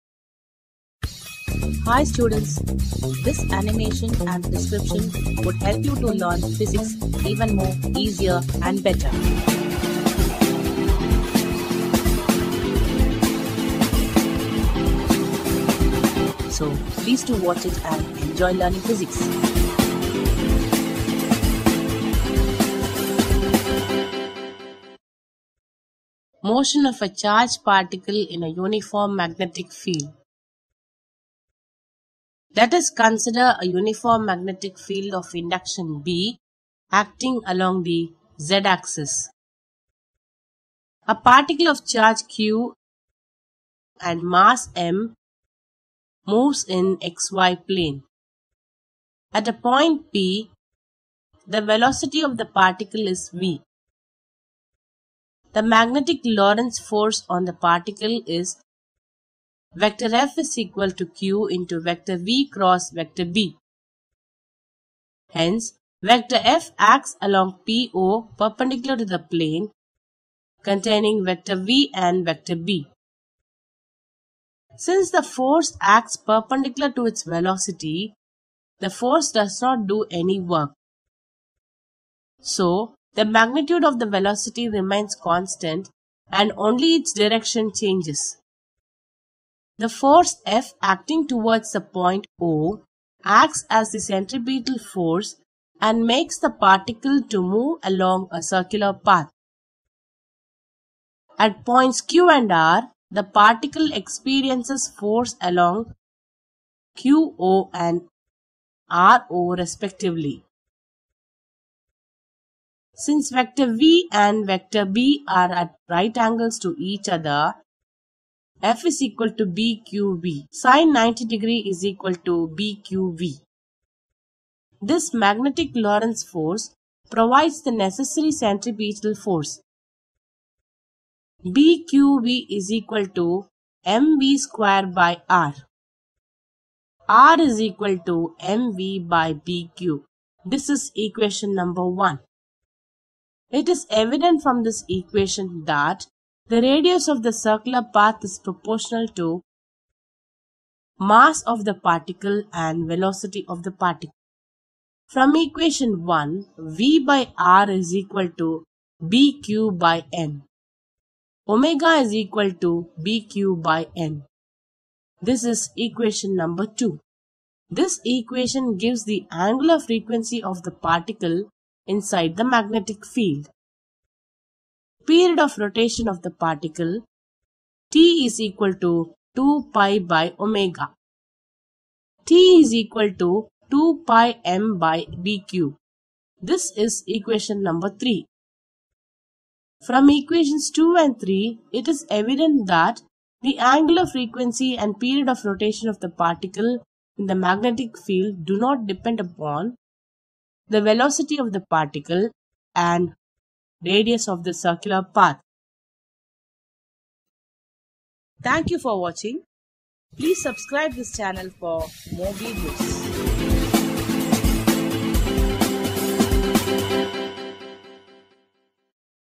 Motion of a Charged Particle in a Uniform Magnetic Field | Physics4students
This physics animation explains Motion of a Charged Particle in a Uniform Magnetic Field. Let us consider a uniform magnetic field of induction B acting along the Z-axis. A particle of charge Q and mass M moves in XY plane. At a point P, the velocity of the particle is V. The magnetic lorentz force on the particle is vector F is equal to Q into vector V cross vector B.

It is evident from this equation, that the radius of the circular path is
proportional to
(i) mass of the particle and
(ii) velocity of the particle

From equations (2) and (3), it is evident that the angular frequency and period of rotation of the particle in the magnetic field do not depend upon
(i) the velocity of the particle and
(ii) radius of the circular path.